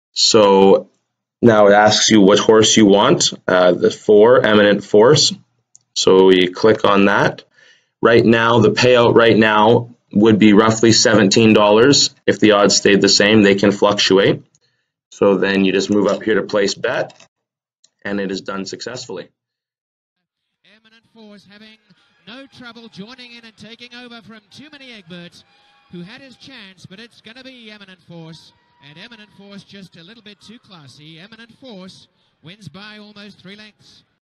BetShare Horse Racing: Woodbine Opening Day Race 2 (April 15, 2017)
Hitting the pavement to get you the best picks.

Jordan's pick: 4
Result: 4, 1, 5
$1 Payout: $3.70